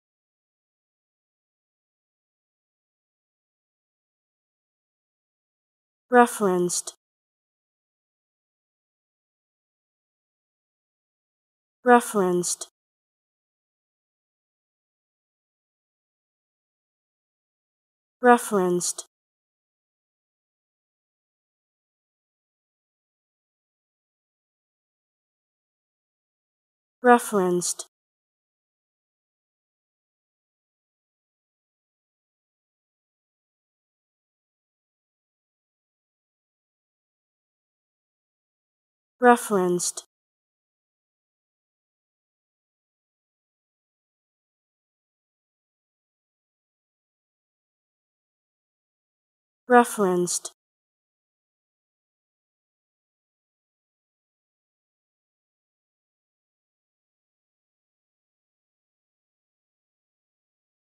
How To Say Referenced
Learn how to say Referenced with EmmaSaying free pronunciation tutorials.